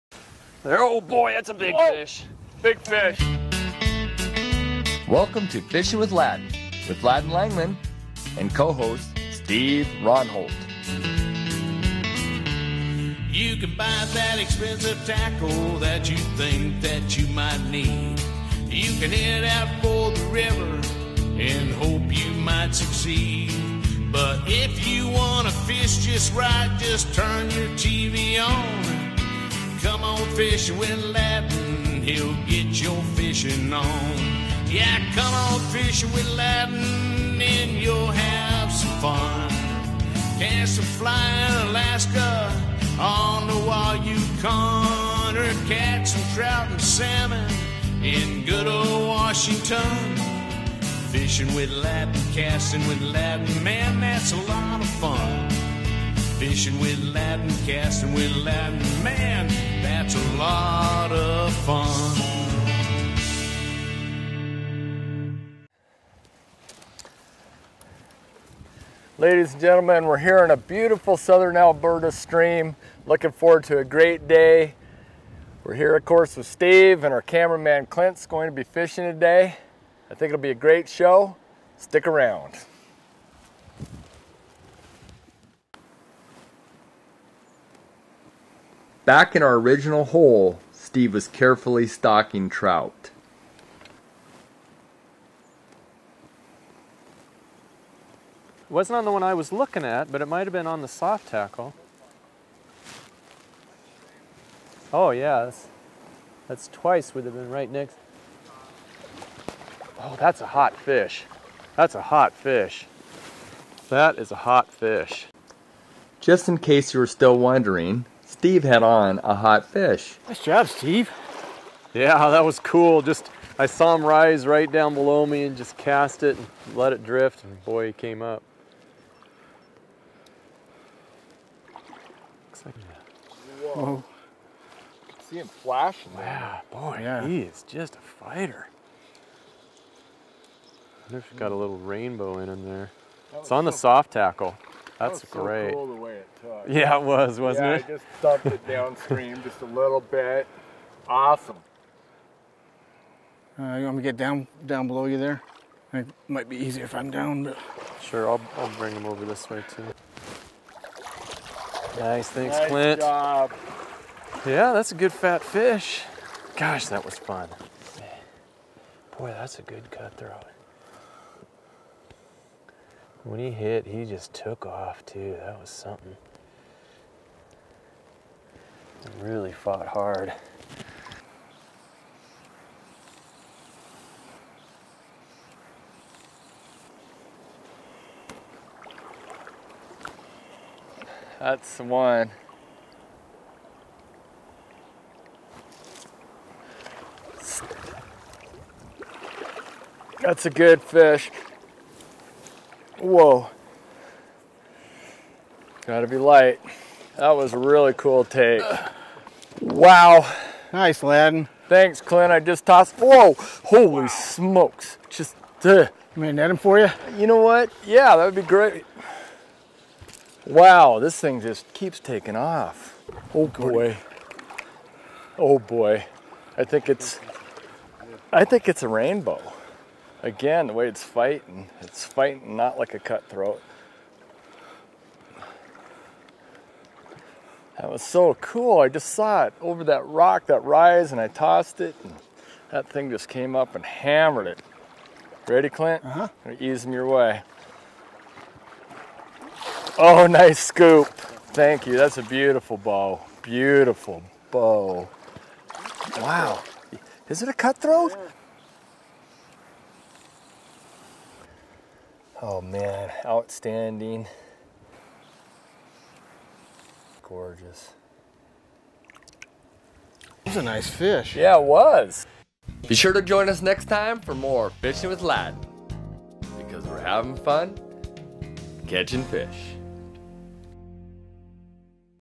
Fly Fishing Alberta Canada Oldman River-Trailer for Prime Video
We are fly fishing Alberta Canada on the crystal-clear waters of the Oldman River, using dry flies and emergers to catch and release remarkable Cutthroat and Rainbow trout.